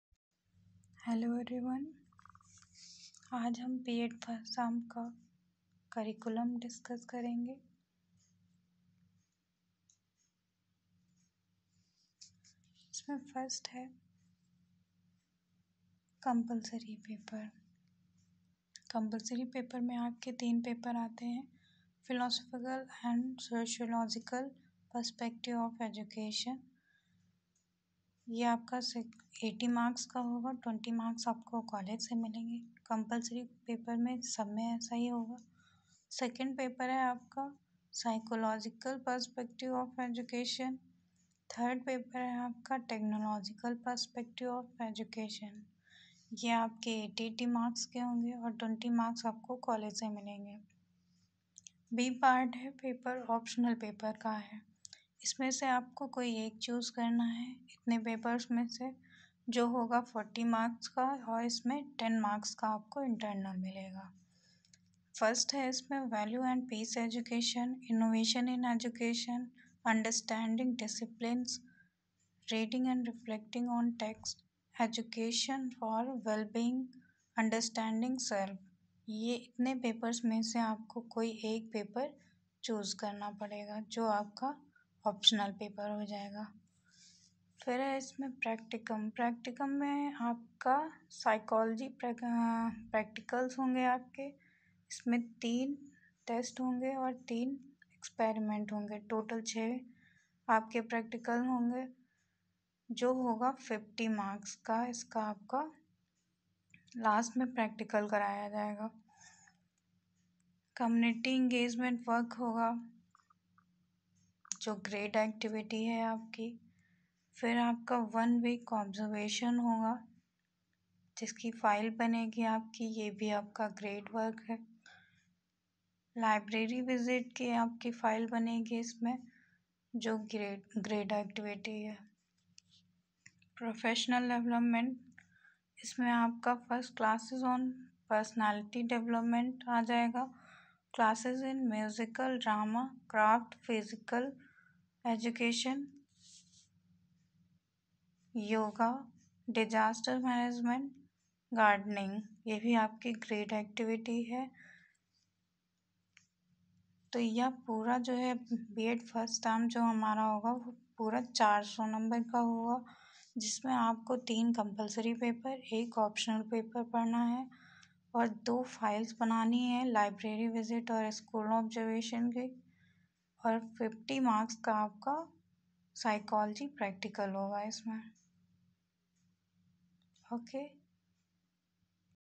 B.Ed. first semester curriculum Discussion
B.Ed first semester paper name & activities whole curriculum